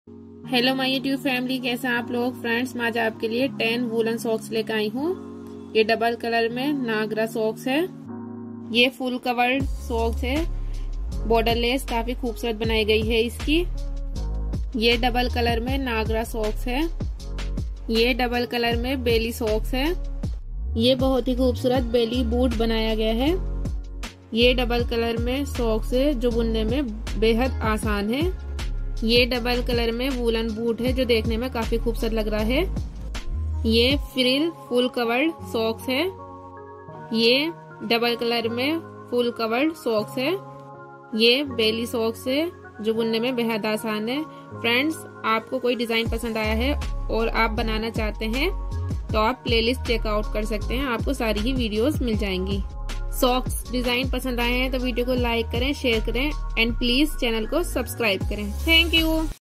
10 woolen socks design/new design socks/woolen boot design for ladies/moje ki design/jurab ka design
10 woolen socks design/new design socks/woolen boot design for ladies/moje ki design/jurab ka design

 Hello friends
i uploads crochet and knitting  skills videos

about video
in this video you watching 10 beautiful design of woolen sicks
these socks is very easy and beautiful
I hope you like this socks designs